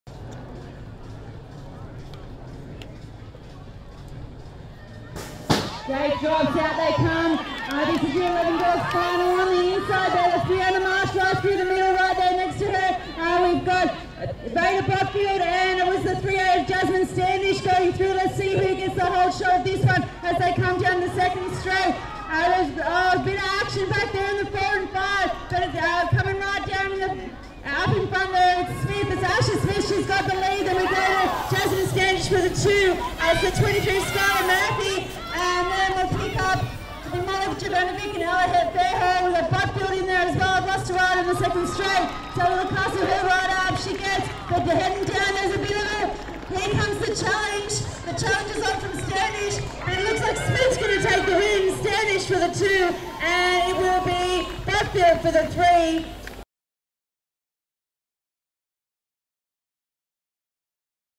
11 Girl's FINAL: STAGE 1, Sydney BMX Club
There was a Queensland trio at the top of the 11-girl's final! 😍

Atherton's Asha Smith just got to the line ahead of Jasmine Standish and Vada Botfield.